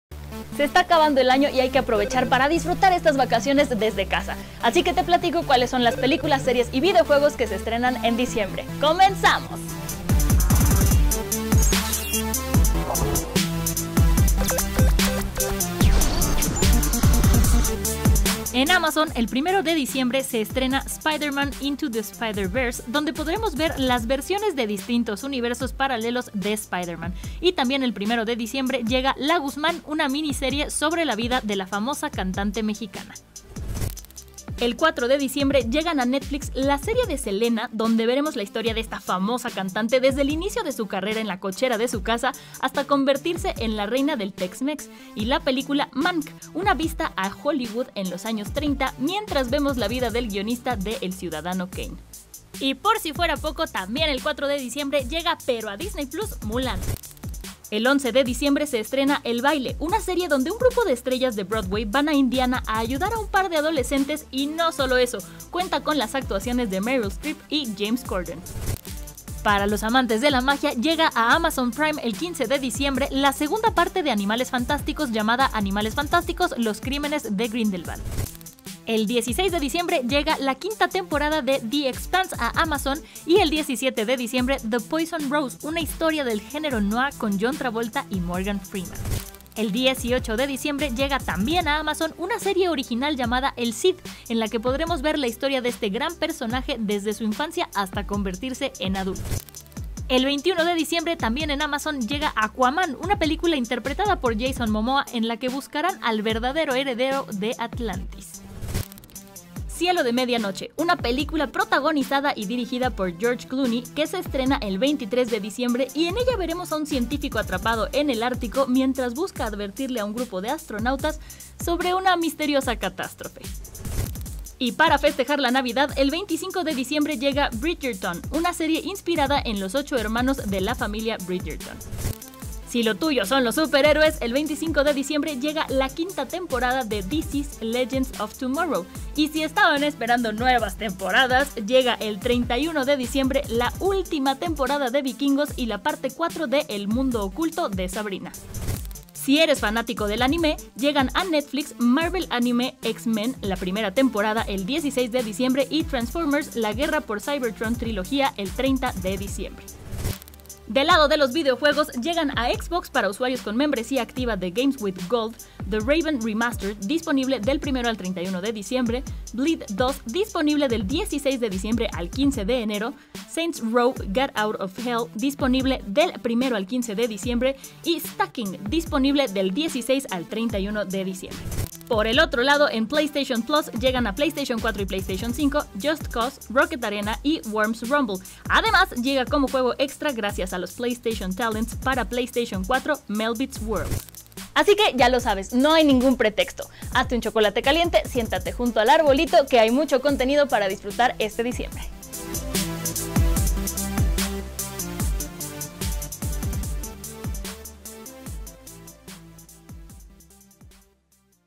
Estrenos diciembre 2020 de Amazon Prime Video, Netflix, DisneyPlus, Xbox y PlayStation
Se acabó el año y hay que aprovechar para descansar en casa, así que prepárate un chocolate caliente para ver todas las series y películas que llegan este mes de diciembre.